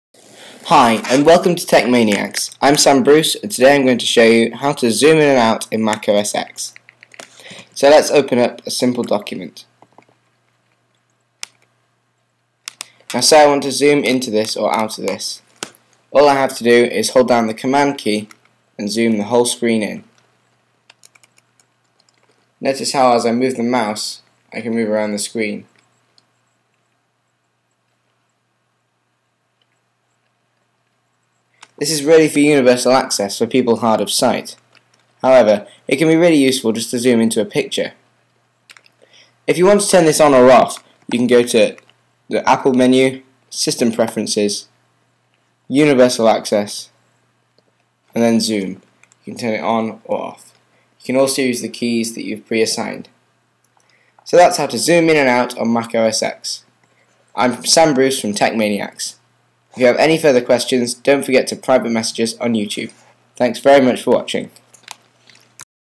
Mac Trick: Quickly zoom in to any part of the screen - Techmaniacs
This isa quick video showing you how to quicly zoom into any part of the screen and move around. This can be useful for people with impared sight or just day to day use if you need to zoom into something!